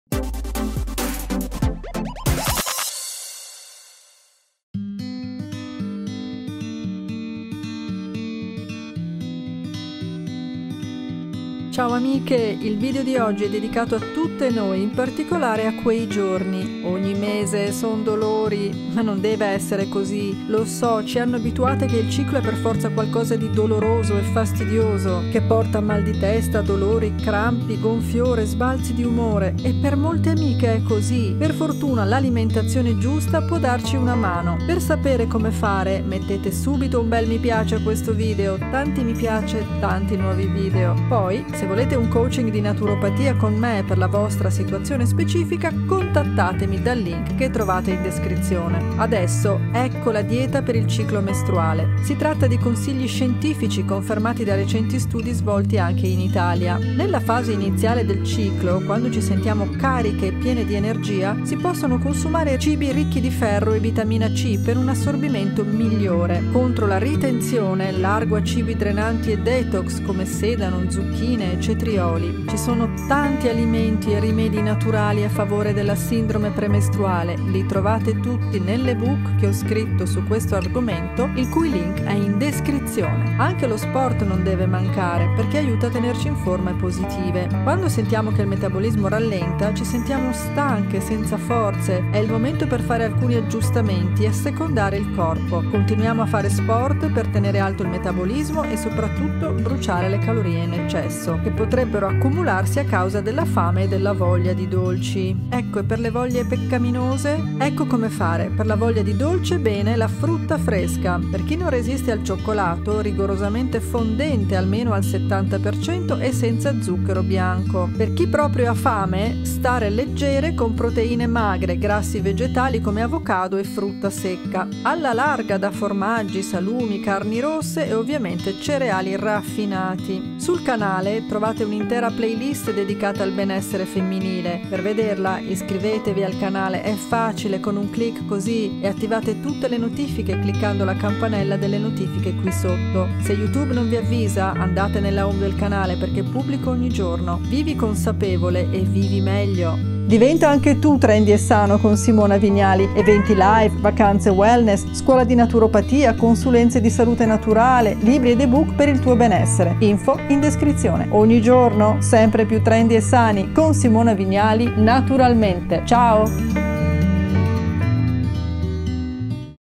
DIETA per PMS, CICLO e DISTURBI MESTRUALI? Ecco come fare!
Ciao amiche! Oggi vi svelo i sedreti della dieta per la PMS secondo i più recenti studi scientifici per contrastare i sintomi della sindrome premestruale.

 EBOOK NOVITA'!!
Addio PMS e SINTOMI PREMESTRUALI

Nota bene

Simona Vignali